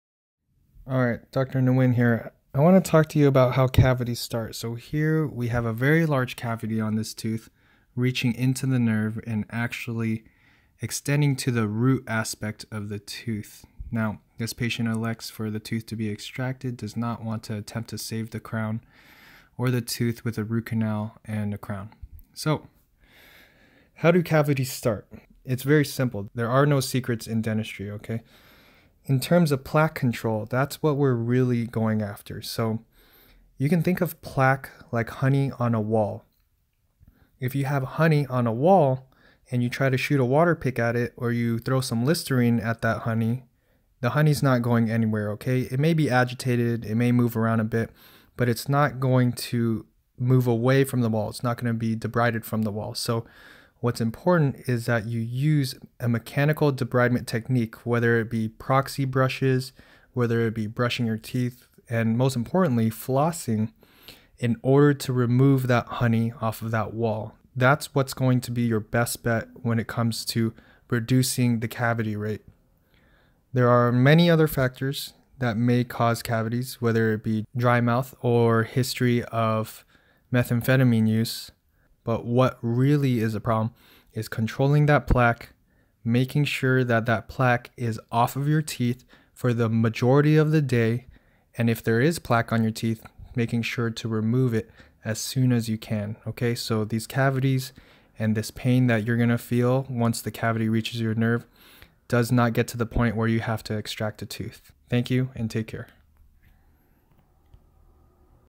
How Cavities Start
Simply put - plaque control is the most important aspect of preventing cavities. There are many reasons as to how a cavity can start, but here we discuss how plaque is like "honey on a wall" and how mechanical removal and debridement is of utmost importance when it comes to preventing cavities.